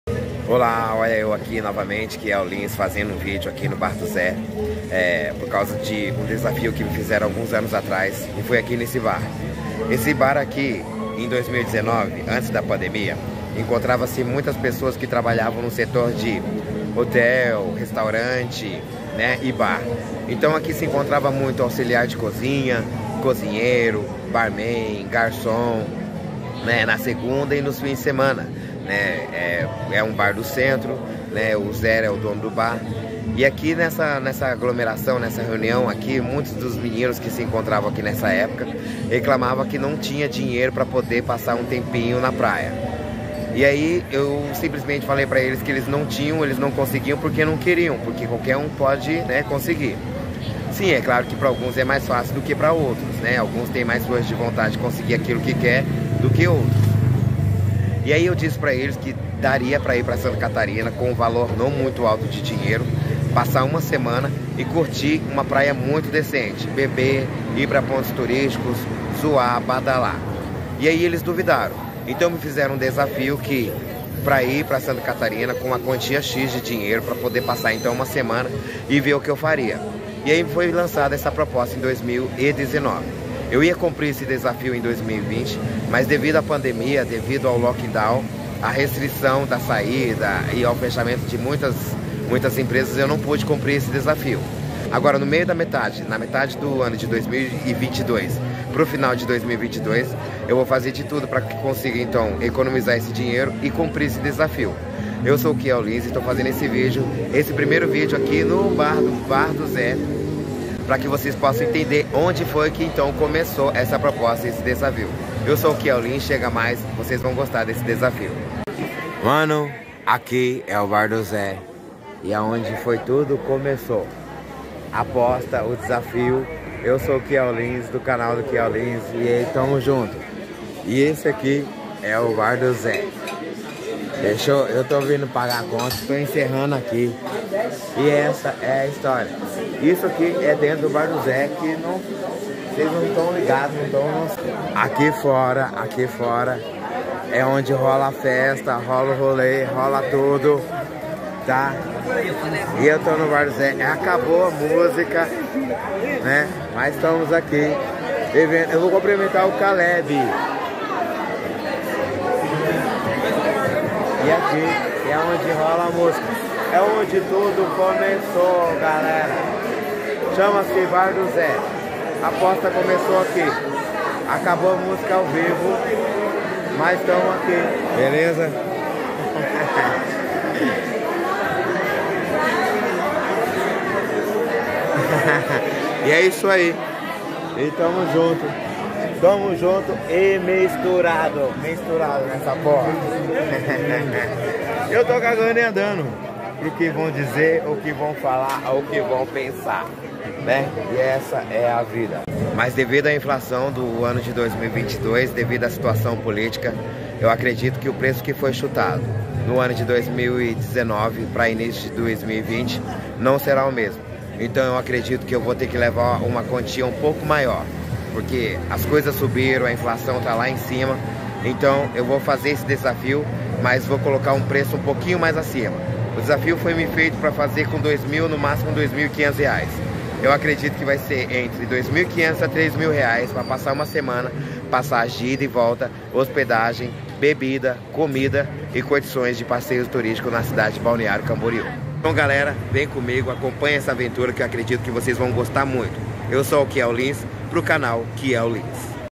Apresentação e Desafio (Kiel Lins - Maringá/PR x Balneário Camboriú/SC) 2022.
Auto-apresentacao, Kiel Lins, descrevendo-me, e o porque do canal no YouTube, falando de mim, a cidade onde moro, Maringá-PR, e o desafio a ser cumprido na cidade de Balneário Camboriú-SC, no Bar do Zé, onde toda essa ideia começou. O desafio a ser cumprido é o de sair de Maringá com uma quantia "X" de dinheiro e ver o que consigo fazer, curtir, usufruir durante uma semana na cidade de Balneário Camboriú. Mas antes de cumprir esse desafio vou me apresentar ao público. Sejam bem vindos (as) ao Canal "Kiel Lins ".